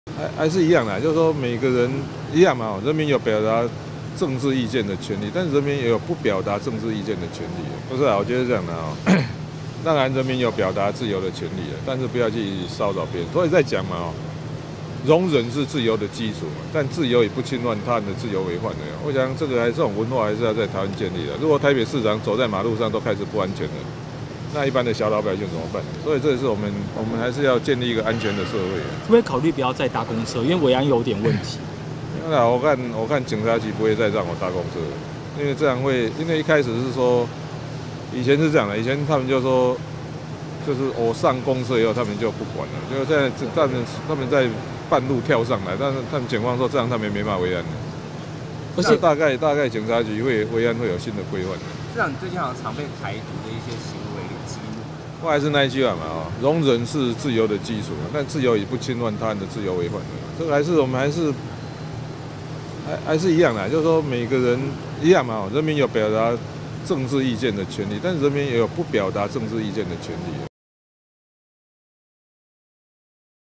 台灣國突擊談世大運維安 柯文哲：人民有不表達意見的權利
☛即新聞粉絲專頁
☛合作洽談信箱 （或私訊即新聞粉絲專頁）